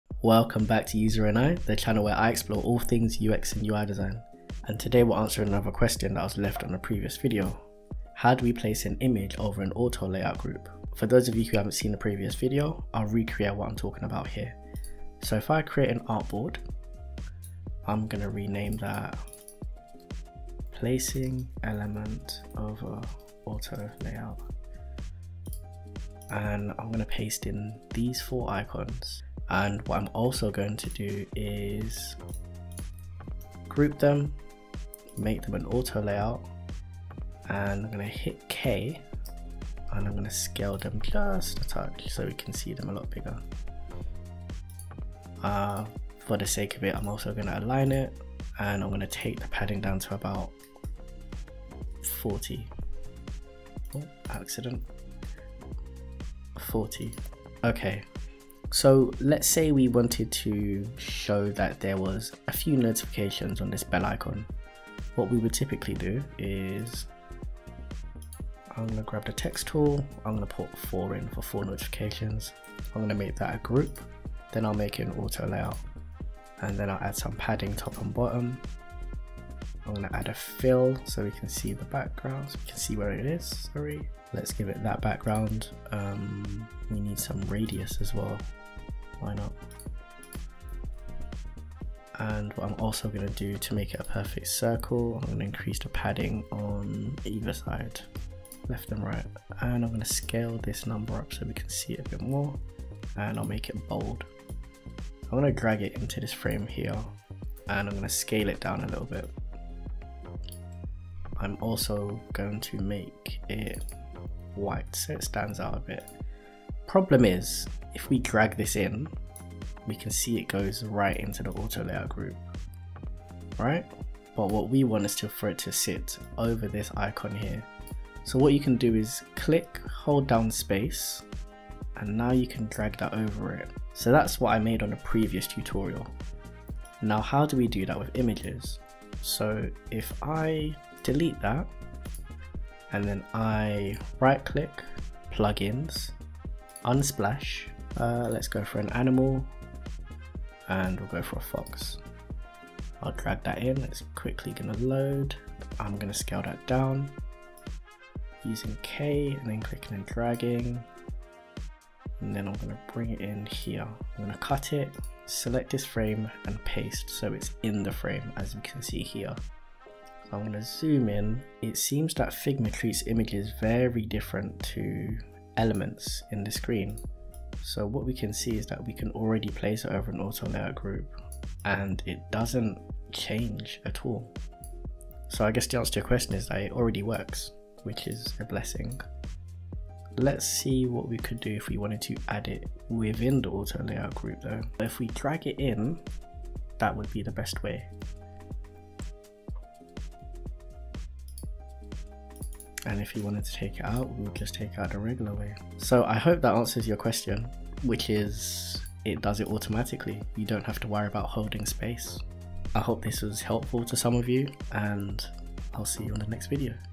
Figma Tip: Placing images over an Auto Layout group | UX/UI
I created a previous video where i explained how to place elements over an auto layout group and this video is all about placing images over and auto layout group.